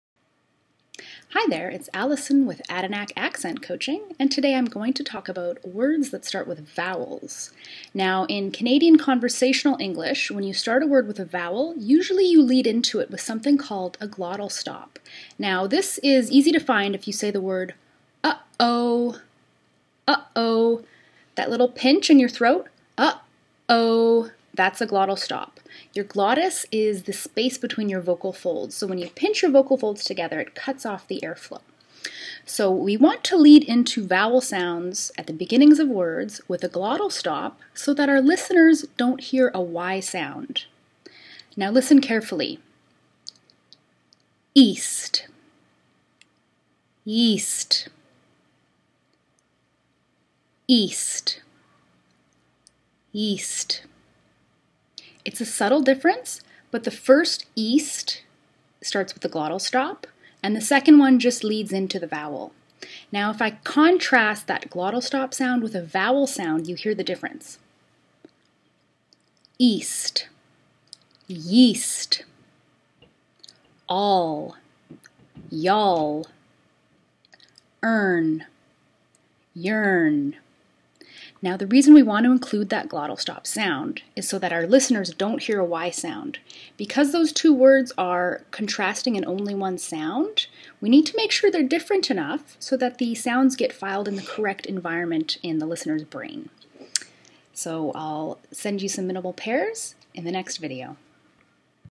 Glottal Stop: location and use (PNW Canadian Accent)
In North American English, words that begin with vowels tend to be started with an invisible sound, called a glottal stop. We use the glottal stop like a consonant.

In North American English, we have a phonological rule that we prefer to lead into vowel sounds with consonants. This is obvious in liaison between words like "took off". In fluent speech, we lead into the "off" with a k- sound. The same goes for leading into syllables that begin with a vowel within a word, such as "determine" or [də.ˈtɝ.mɪn] we use the -t- to lead into the rest of the word.

So, to make a short story long, leading into your vowel words with a glottal stop will sound more native-like.